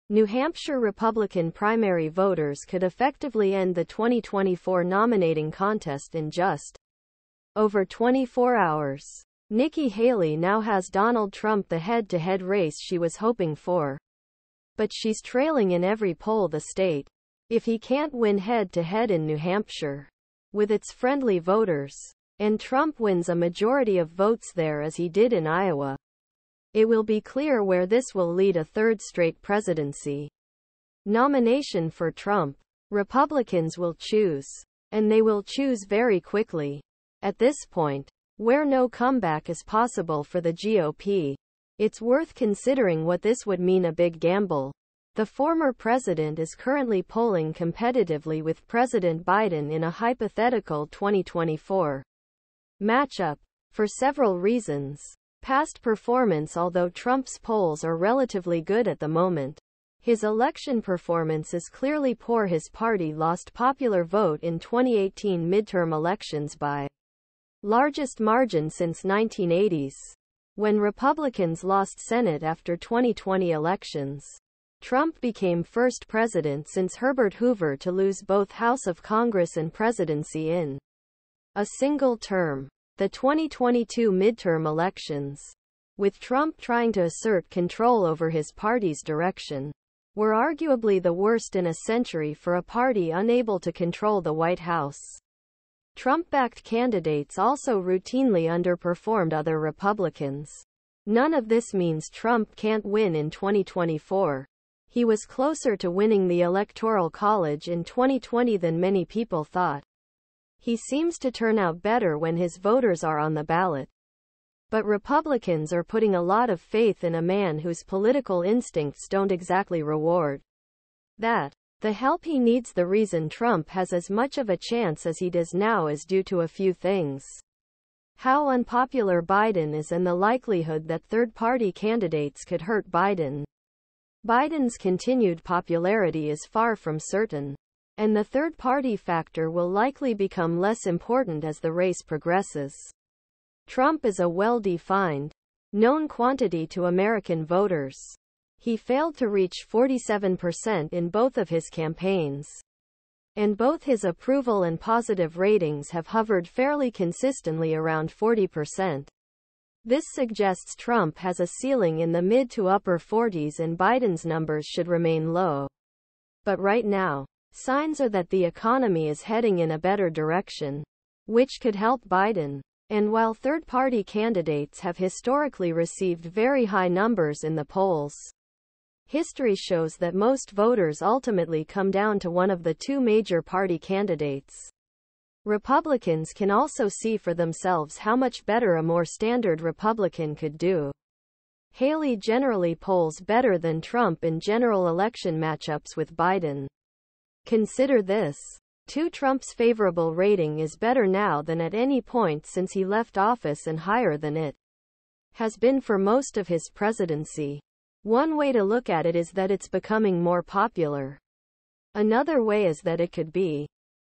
4 reasons Trump is a gamble for Republicans
New Hampshire Republican primary voters could effectively end the 2024 nominating contest in just over 24 hours. Nikki Haley now has Donald Trump the head-to-head race she was hoping for, but she's trailing in every poll the state. If he can't win head-to-head in New Hampshire, with its friendly voters, and Trump wins a majority of votes there as he did in Iowa, it will be clear where this will lead A third straight presidency. Nomination for Trump. Republicans will choose, and they will choose very quickly. At this point, where no comeback is possible for the GOP, it's worth considering what this would mean a big gamble. The former president is currently polling competitively with President Biden in a hypothetical 2024 matchup. For several reasons. past performance Although Trump's polls are relatively good at the moment, his election performance is clearly poor His party lost popular vote in 2018 midterm elections by largest margin since 1980s. When Republicans lost Senate after 2020 elections, Trump became first president since Herbert Hoover to lose both house of Congress and presidency in a single term. The 2022 midterm elections, with Trump trying to assert control over his party's direction, were arguably the worst in a century for a party unable to control the White House. Trump-backed candidates also routinely underperformed other Republicans. None of this means Trump can't win in 2024. He was closer to winning the electoral college in 2020 than many people thought. He seems to turn out better when his voters are on the ballot. But Republicans are putting a lot of faith in a man whose political instincts don't exactly reward that. The help he needs The reason Trump has as much of a chance as he does now is due to a few things how unpopular Biden is and the likelihood that third-party candidates could hurt Biden. Biden's continued popularity is far from certain, and the third-party factor will likely become less important as the race progresses. Trump is a well-defined, known quantity to American voters. He failed to reach 47 percent in both of his campaigns, and both his approval and positive ratings have hovered fairly consistently around 40 percent. This suggests Trump has a ceiling in the mid-to-upper 40s and Biden's numbers should remain low. But right now, signs are that the economy is heading in a better direction, which could help Biden. And while third-party candidates have historically received very high numbers in the polls, history shows that most voters ultimately come down to one of the two major party candidates. Republicans can also see for themselves how much better a more standard Republican could do. Haley generally polls better than Trump in general election matchups with Biden. Consider this, too Trump's favorable rating is better now than at any point since he left office and higher than it has been for most of his presidency . One way to look at it is that it's becoming more popular; Another way is that it could be